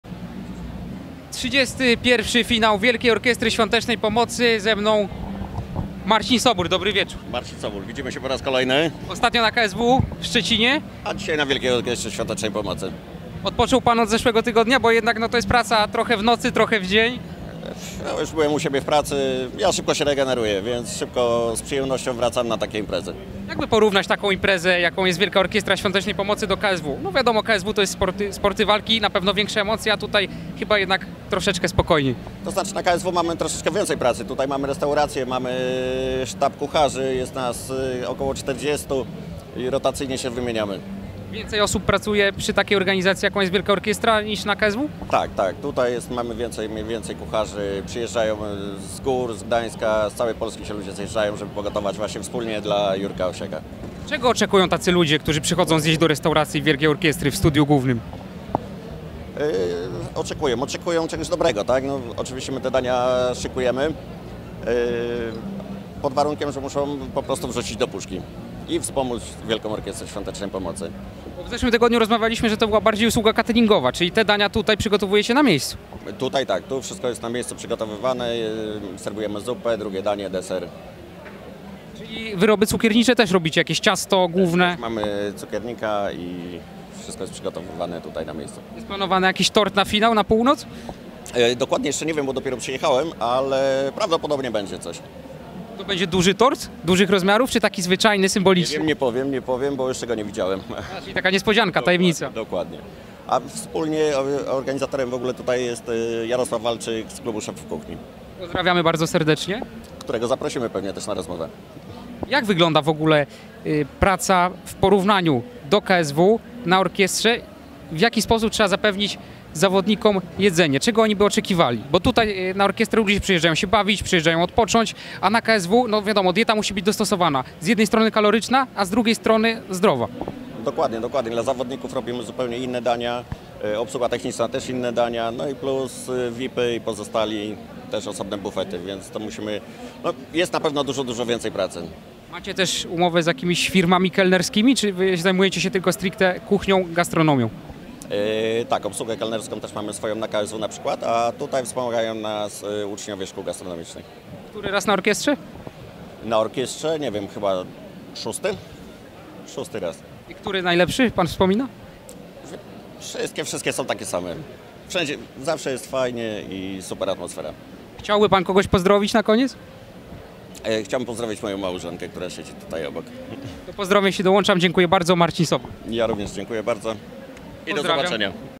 WCZEŚNIEJ KSW TERAZ WOŚP MARCIN SOBÓL - Warszawa 29.01.2023, Orkiestra
Kucharz MARCIN SOBÓL - finał 31 WOŚP - Warszawa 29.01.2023, Orkiestra Świątecznej Pomocy
channelsporty, sporty channel, tv foto,
wośp, wielka orkiestra świątecznej pomocy, 17 wośp,